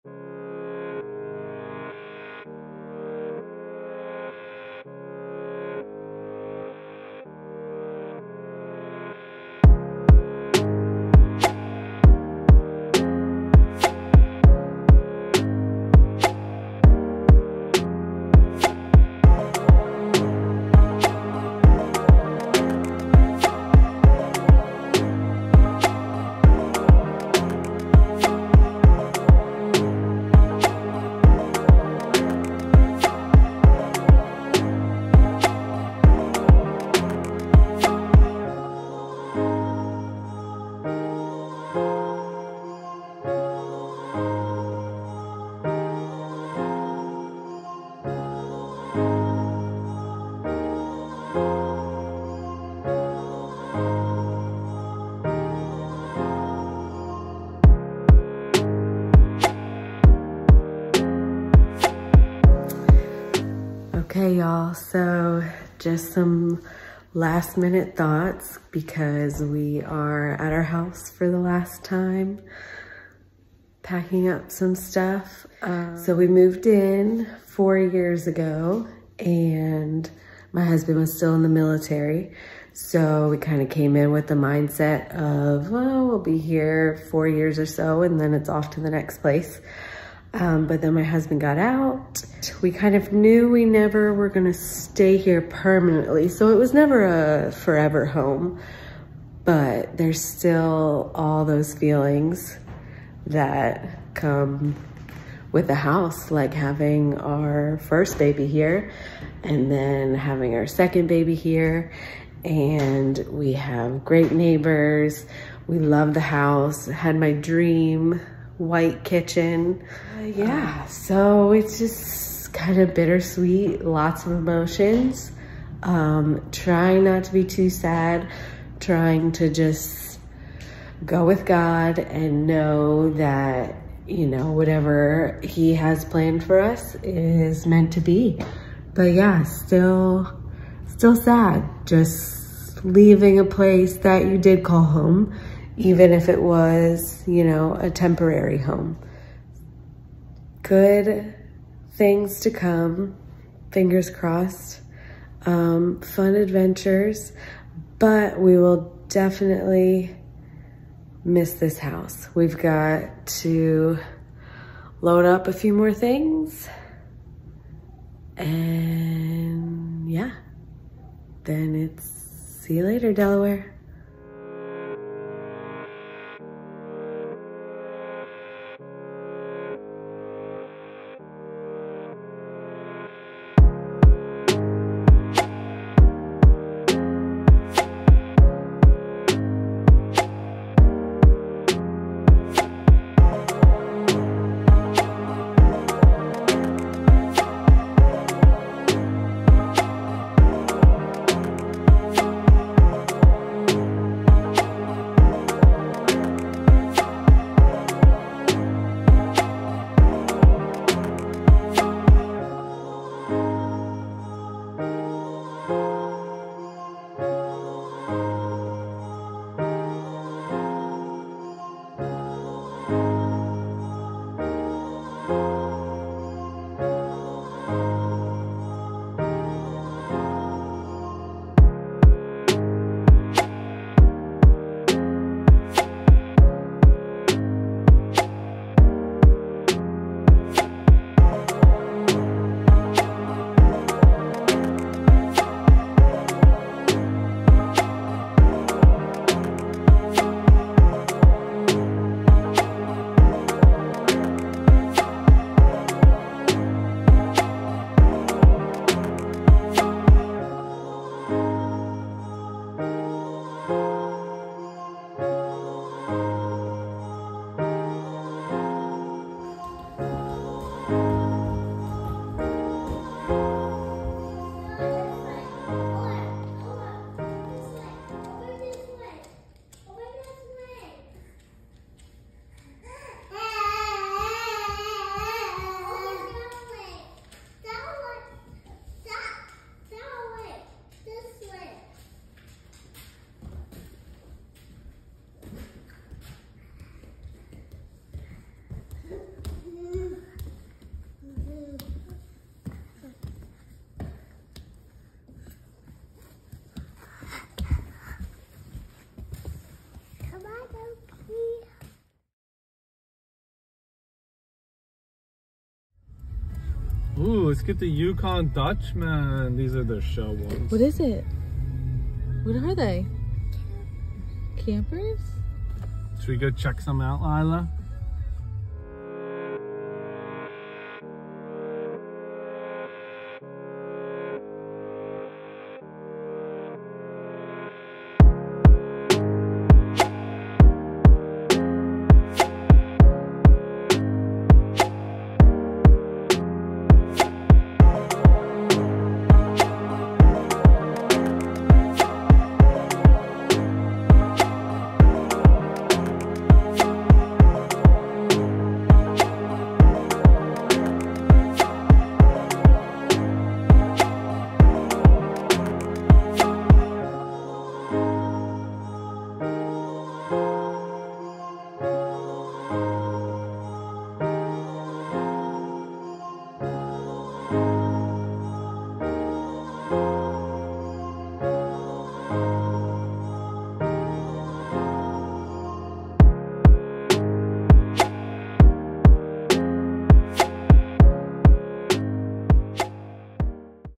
RV LIFE / The Beginning of our Journey / From Brick & Mortar to a Home on Wheels
We are the Viguers and in February of 2023 we decided to sell our house, buy an RV, and hit the road with our 2 kiddos and pup. Here is a look back at our journey of packing up our first family home in Delaware, some last minute thoughts, and the search for our RV. Follow along with us & enjoy our journey!